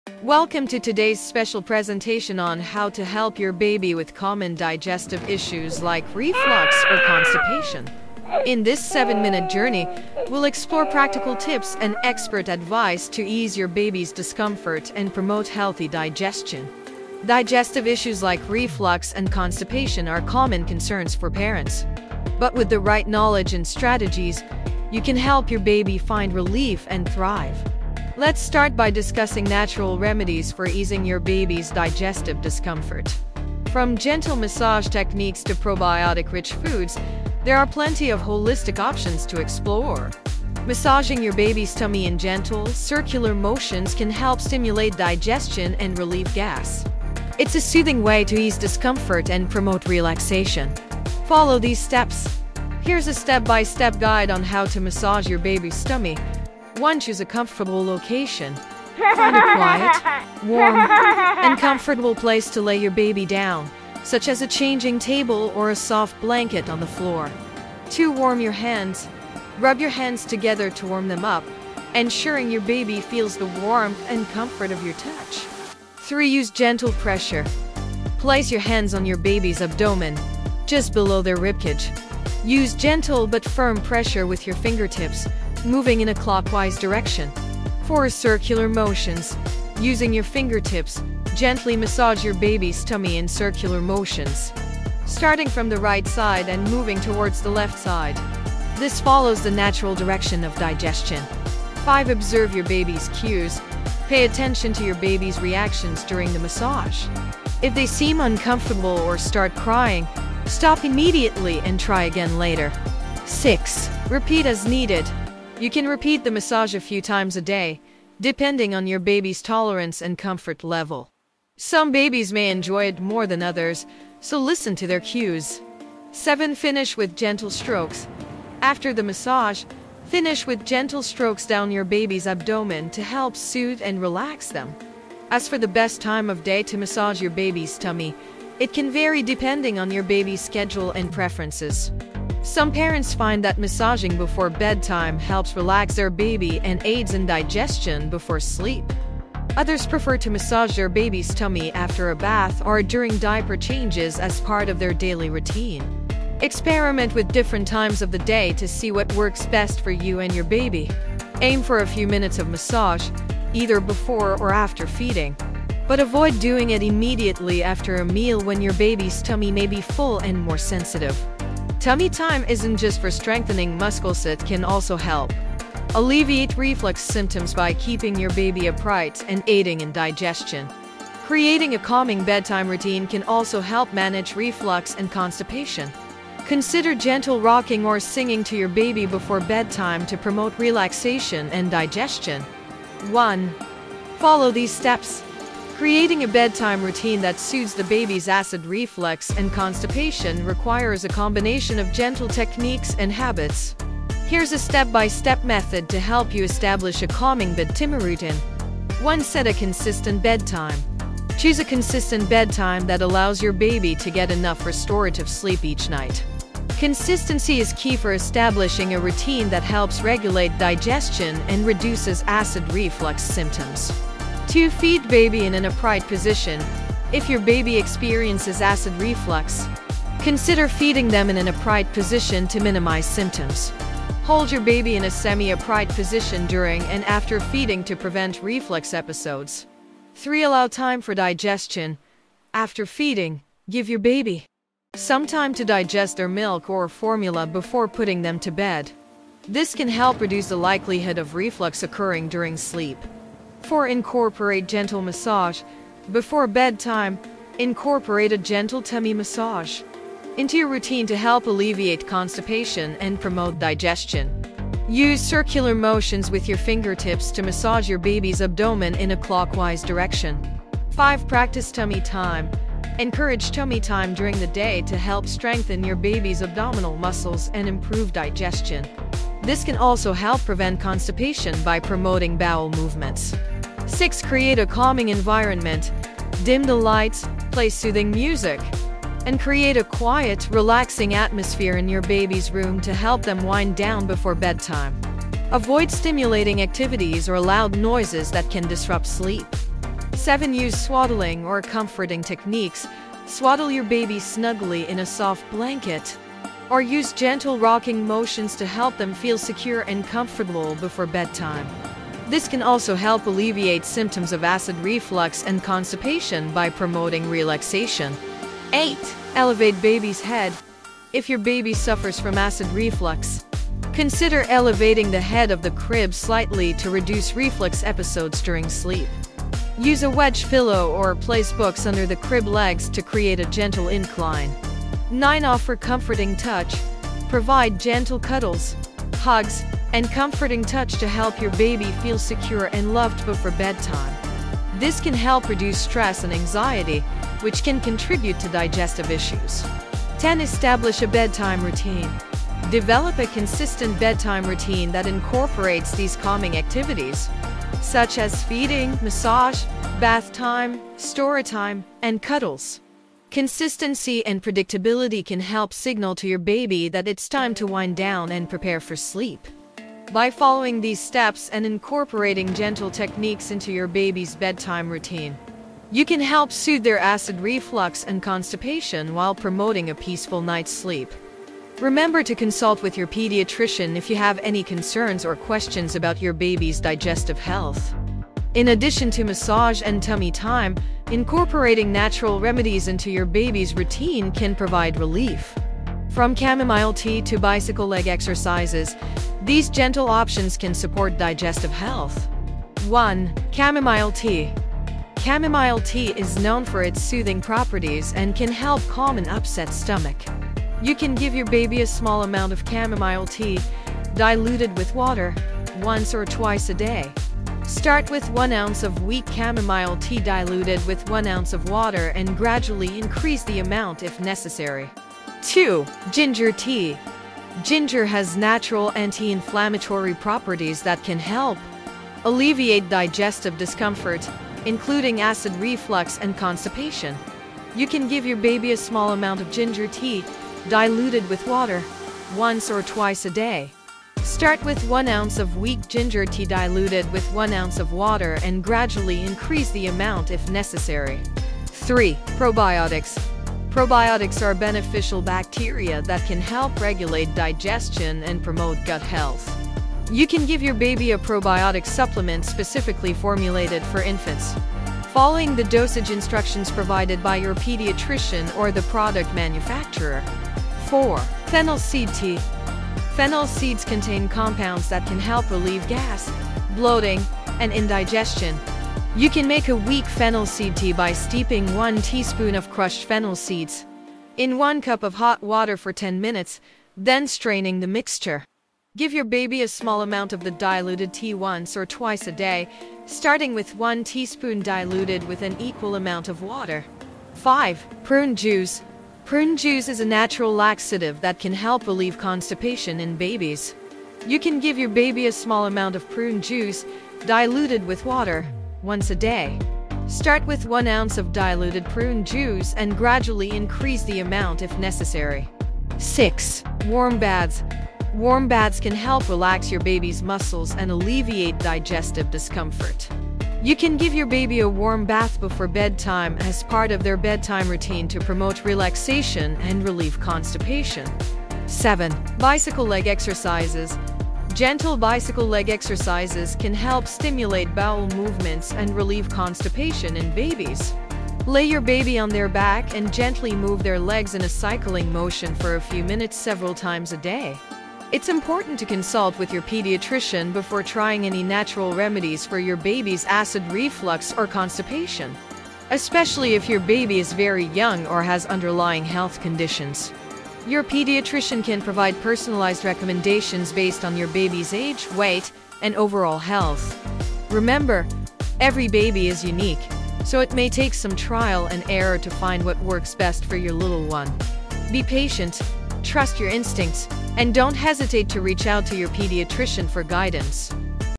Comprehensive Guide To Fight Constipation and Acid Reflux In Babies
Title: "Natural Remedies for Baby's Acid Reflux and Constipation: Soothing Solutions for Digestive Discomfort"

Description:
👶🍵 Seeking gentle relief for your baby's digestive woes? Join us for a comprehensive guide on natural remedies to alleviate acid reflux and constipation in infants. From soothing chamomile tea to gentle tummy massage techniques, this video offers practical solutions to ease your baby's discomfort naturally. Learn how to incorporate probiotics, fennel seed tea, and warm baths into your baby's routine to promote healthy digestion and bowel movements. Empower yourself with expert advice and gentle remedies to help your little one thrive. Watch now and discover the soothing solutions your baby deserves! 🌿✨